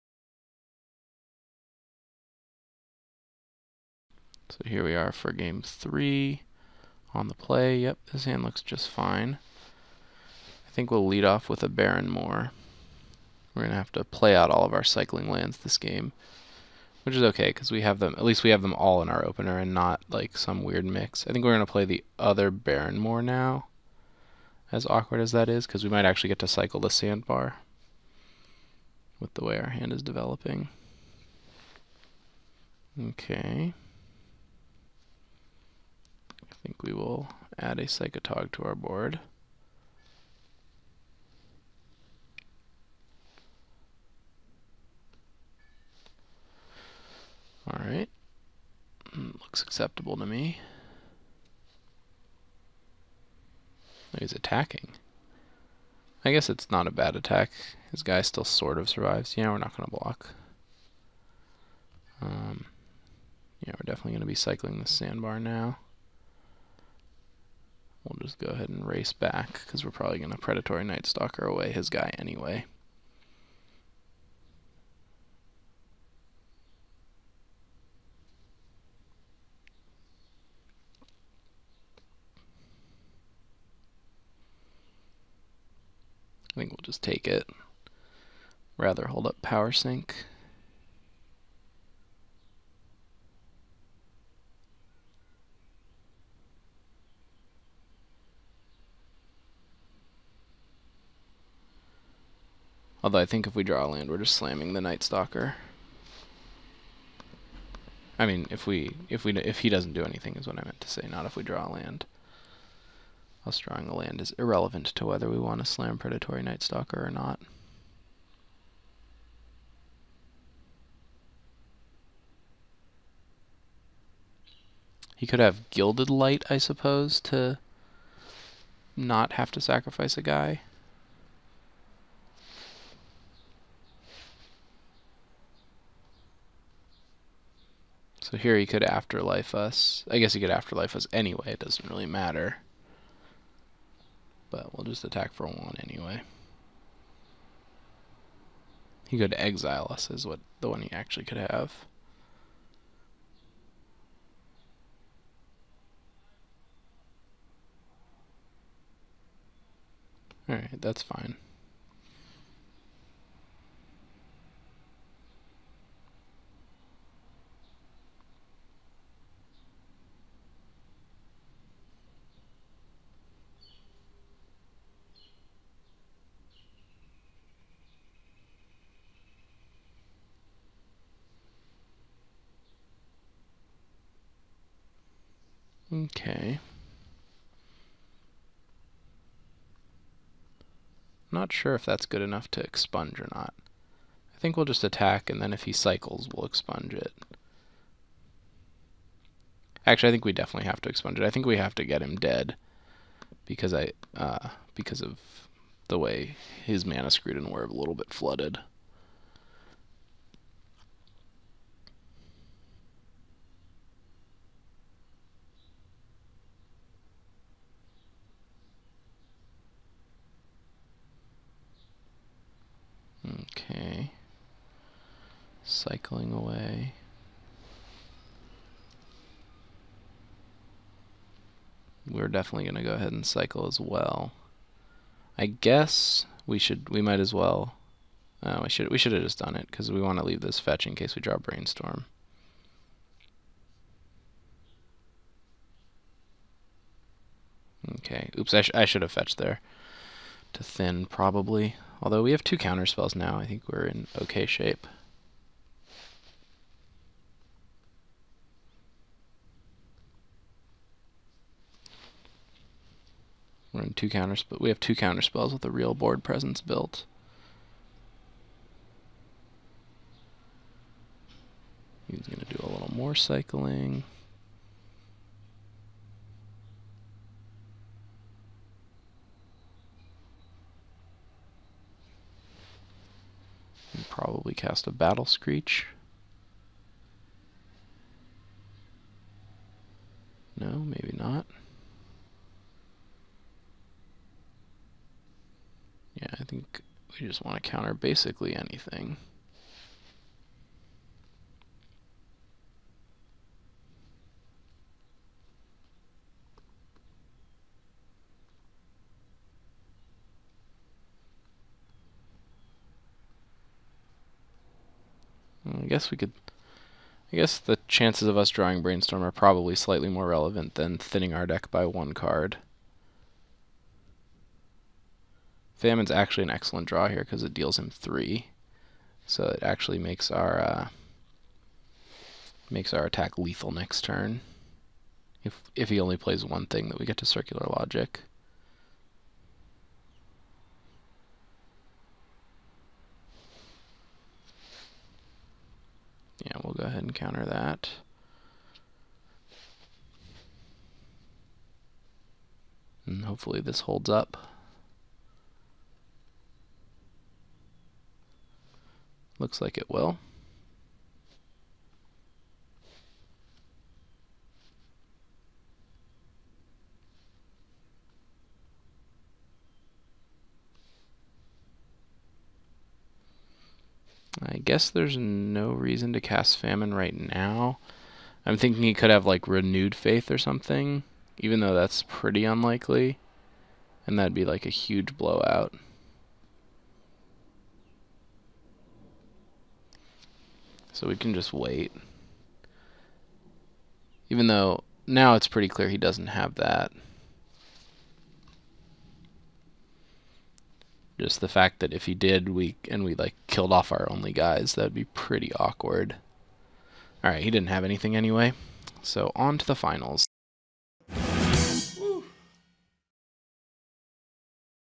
Channel Smdster - Vintage Masters Draft #3 (Match 2, Game 3)
Grand Prix Champion Sam Pardee guides you through a Vintage Masters draft, breaking down each pick and every match!